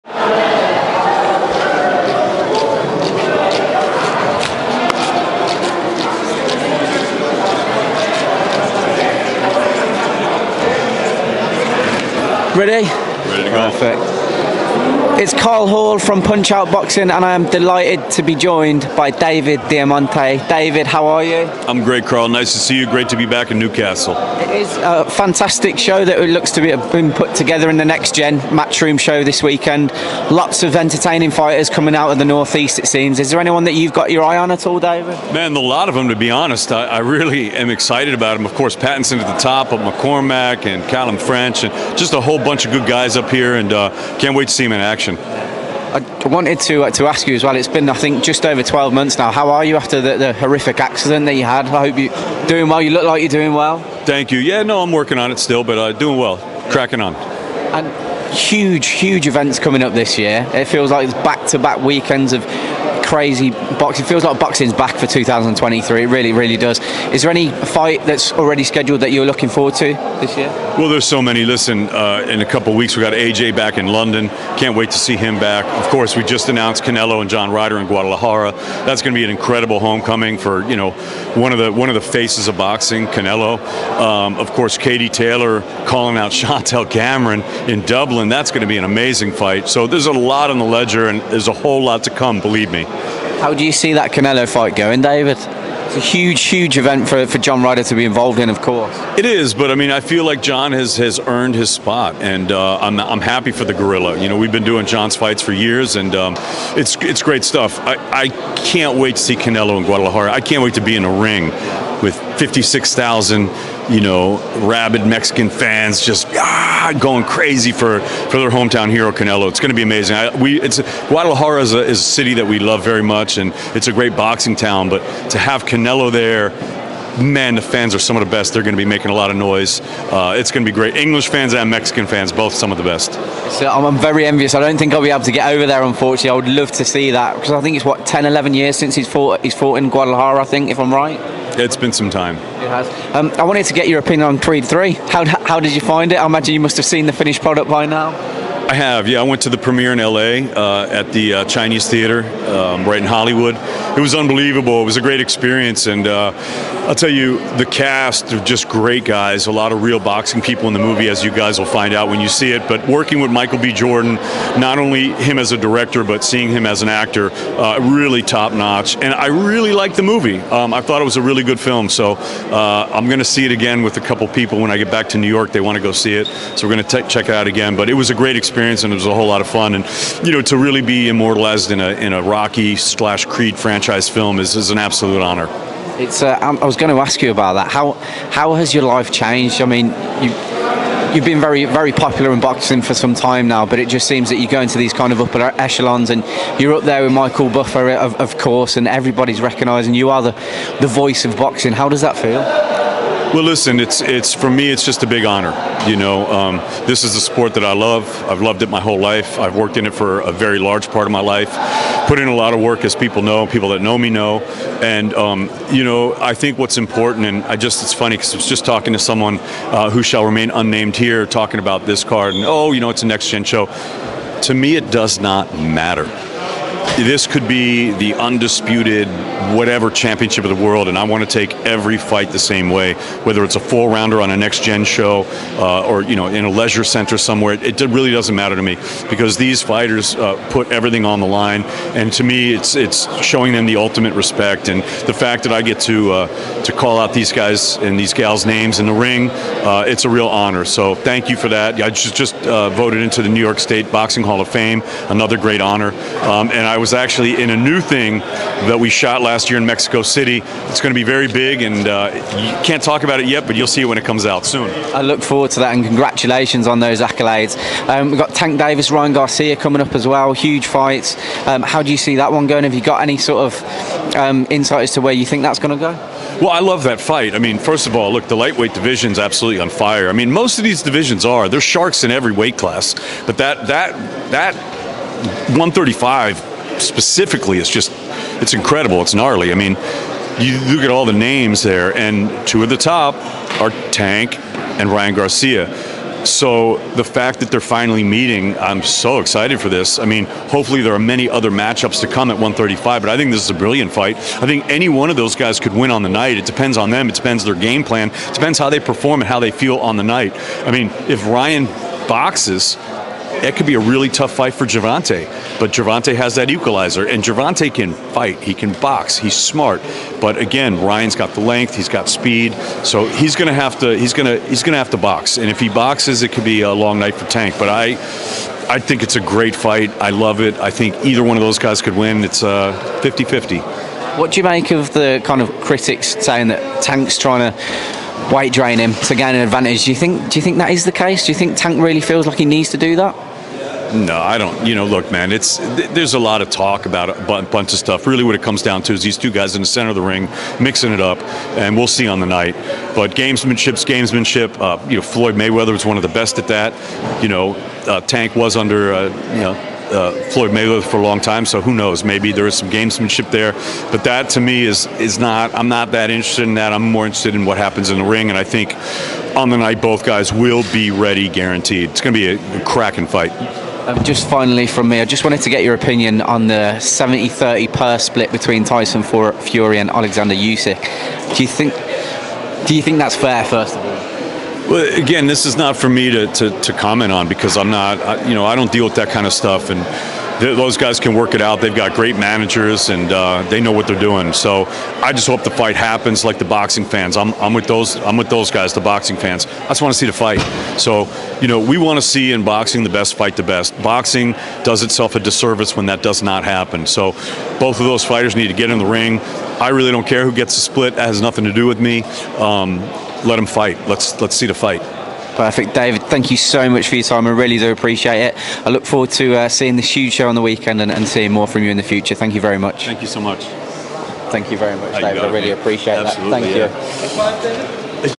JOHN RYDER HAS EARNED HIS SPOT - DAVID DIAMANTÉ ON CANELO/RYDER, FURY/USYK AND CREED 3
'BOXING NEEDS AN UNDISPUTED HEAVYWEIGHT' - DAVID DIAMANTE ON FURY/USYK,

“IM HAPPY FOR THE GORILLA”

“JOHN RYDER HAS EARNED HIS SPOT”

DAVID DIAMANTÉ TALKS ABOUT CREED 3, MICHAEL B JORDAN BEING A FANTASTIC DIRECTOR.
CANELO V RYDER AND FURY V USYK

INTERVIEW BY KARL HALL